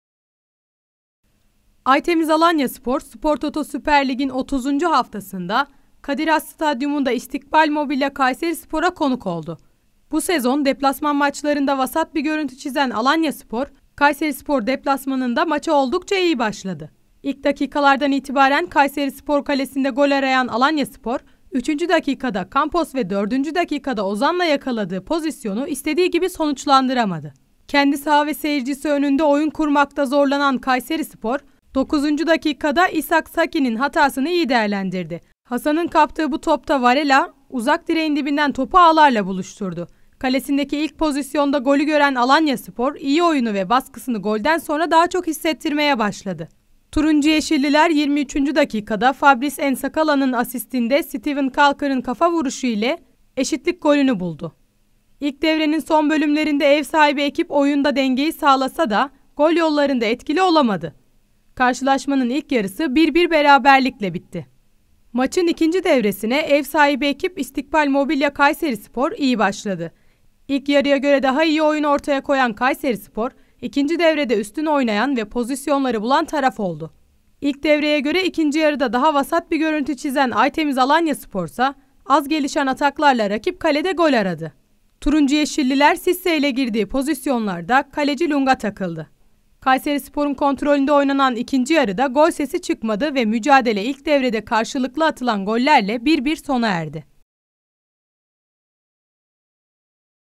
ALANYASPOR 1 PUANA RAZI OLDU
Aytemiz Alanyaspor, 30. hafada deplasmanda karşılaştığı İstikbal Mobilya Kayserispor ile berabere kaldı. Goller maçın ilk devresinde gelirken, Alanyaspor'un beraberlik golünü atan Seteven Caulker, turuncu-yeşilli forma ile ilk golünü attı.

Aytemiz Alanyaspor, Spor Toto Süper Lig'in 30. haftasında Kadir Has Stadyumu'nda İstakbal Mobilya Kayserispor'a konuk oldu. Bu sezon deplasman maçlarında vasat bir görüntü çizen Alanyaspor, Kayserispor deplasmanında maça oldukça iyi başladı. İlk dakikalardan itibaren Kayserispor kalesinde gol arayan Alanyaspor, 3. dakikada Campos ve 4. dakikada Ozan ile yakaladığı pozisyonu istediği gibi sonuçlandıramadı. Kendi saha ve seyircisi önünde oyun kurmakta zorlanan Kayserispor, 9. dakikada İsaac Sackey'in hatasını iyi değerlendirdi. Hasan'ın kaptığı bu topta Varela, uzak direğin dibinden topu ağlala buluşturdu. Kalesindeki ilk pozsiyonfa golü gören Alanyaspor, iyi oyunu ve baskısını golden sonra daha çok hissettirmeye başladı. Turuncu-yeşilliler, 23. dakikada Fabrice N'Sakala'nın asistinde Steven Caulker'in kafa vuruşu ile eşitlik golünü buldu. İlk devrenin son bölümlerinde ev sahibi ekip oyunda dengeyi sağlasa gol yollarında etkili olamadı. Karşılaşmanın ilk yarısı 1-1 beraberlikle geçildi.

Maçın 2. devresine ev sahibi ekip İstikbal Mobilya Kayserispor iyi başladı. İlk devreye göre daha iyi oyun ortaya koyan Kayserispor, ikinci deverde üstün oynayan ve pozisyonları bulan taraf oldu. İlk devreye göre ikinci yarıda daha vasat bir görüntü çizen Aytemiz Alanyaspor ise, az gelişen ataklarla rakip kalede gol aradı. Turuncu-yeşilliler, Cisse ile girdiği pozisyonlarda kaleci Lung'a takıldı. Kayserispor'un kontrolünde oynanan ikinci yarıda gol sesi çıkmadı ve mücadele ilk devrede karşılıklı atılan gollerle 1-1 sona erdi.